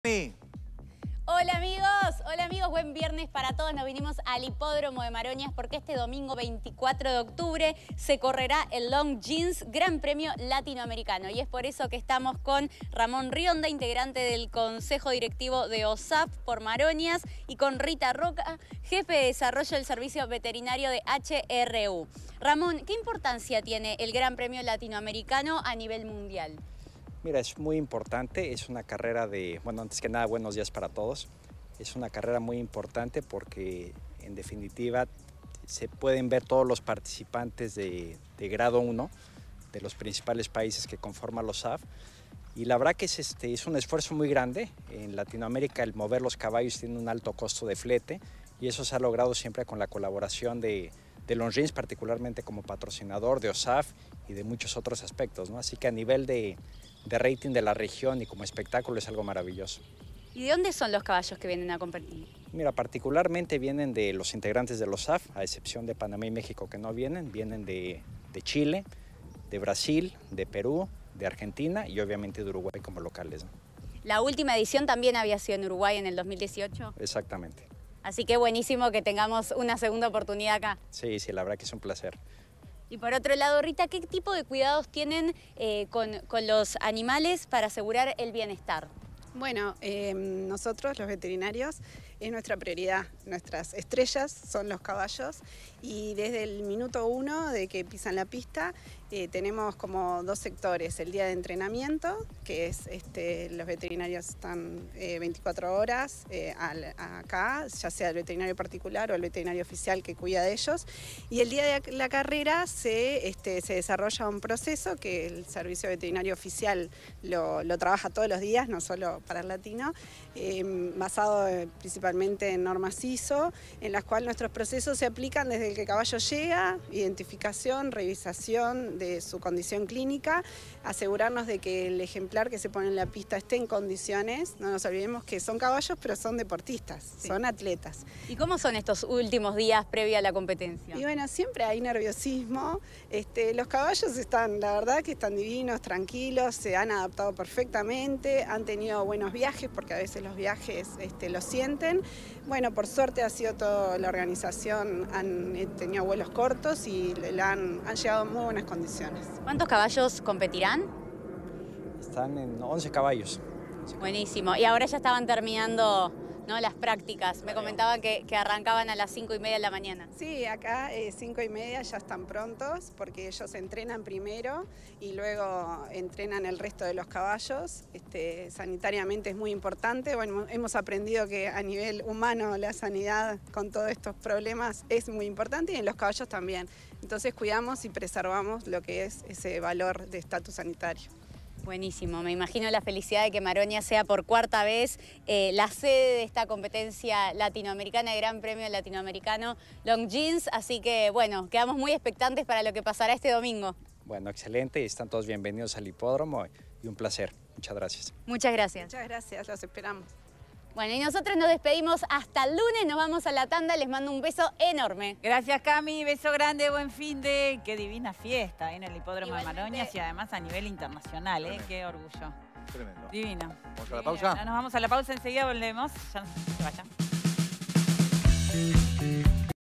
La competencia hípica más importante de Latinoamérica llega a Maroñas
En el móvil de Cami conversamos con Ramón Rionda y Rita Roca, quienes nos contaron sobre la carrera que se realizará en el Hipódromo de Maroñas el próximo 24 de octubre, que incluye los mejores caballos de Argentina, Brasil, Chile, Perú y Uruguay.